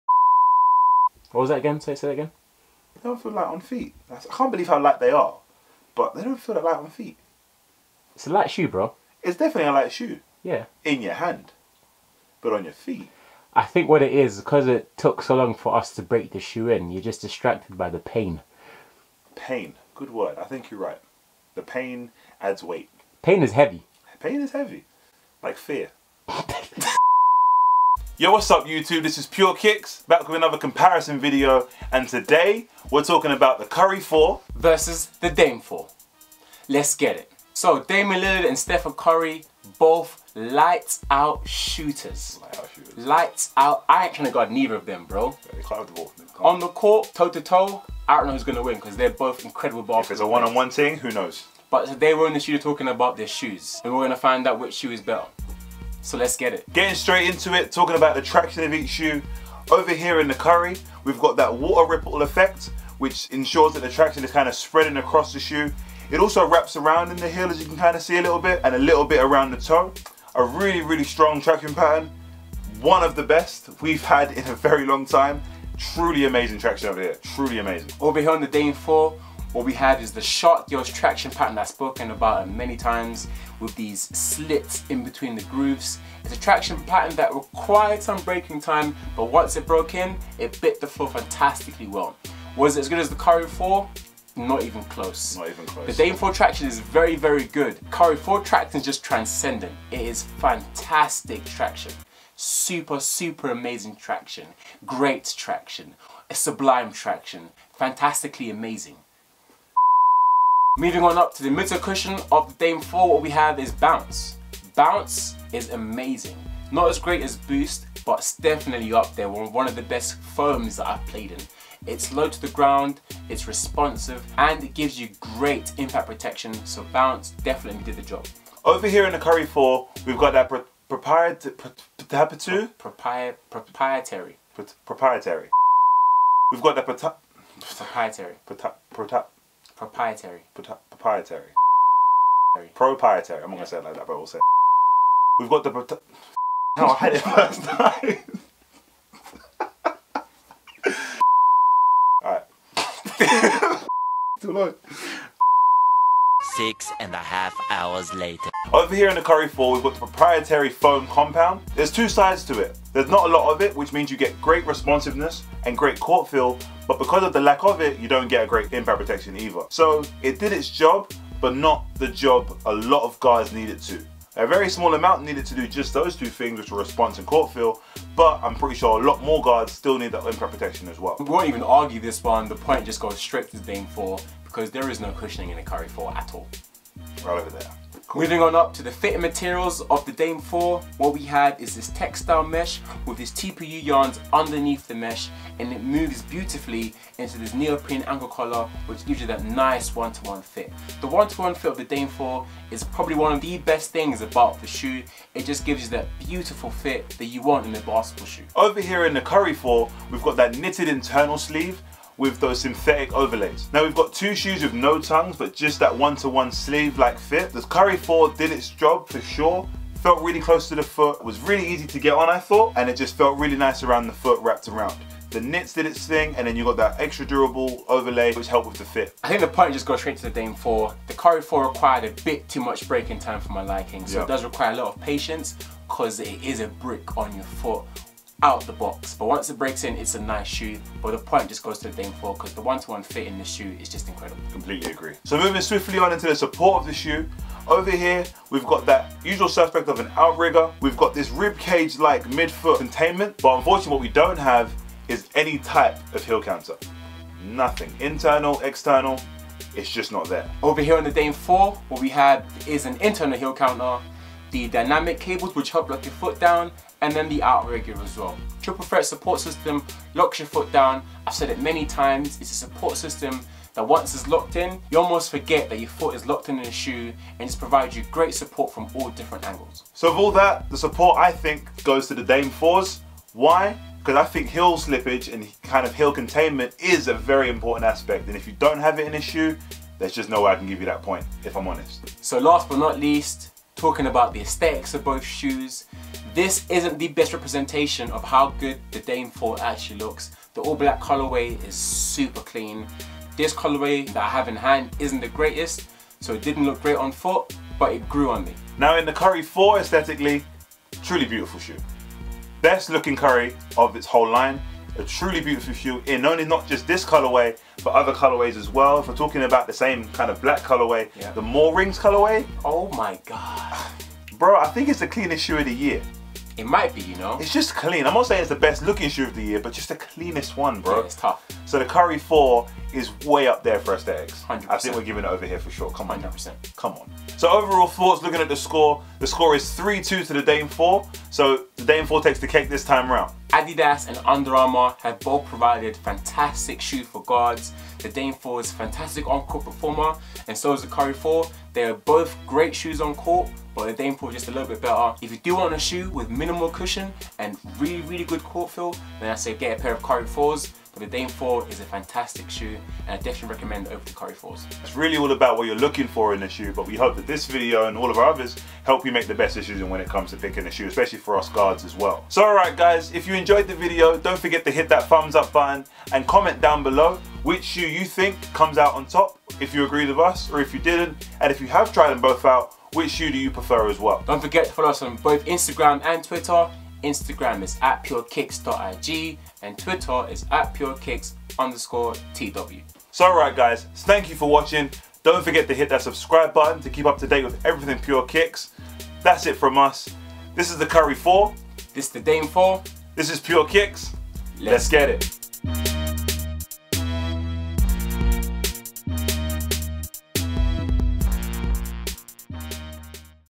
ADIDAS DAME 4 vs UNDER ARMOUR CURRY 4 COMPARISON
We're back with another comparison video, this time we put two of the most electric point guards in the ring to find out who has the better performance shoe!